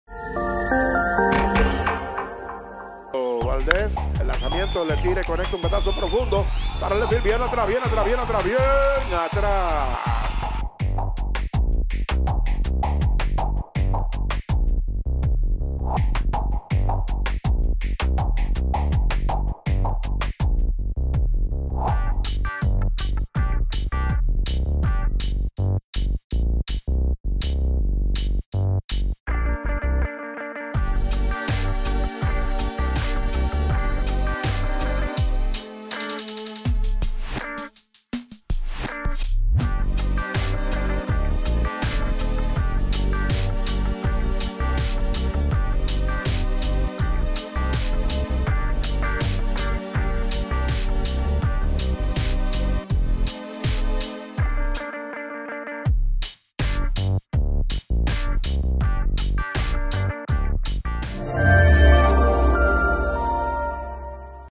Homerun | Jorge Mateo (TE) vs Tigres del Licey | 21 ENE 2020 | Serie Final Lidom
¡Llévate la alegría de tu pelota invernal con DR Sports! ⚾

[Disponible en DRSports.tv, Google Play y App Store]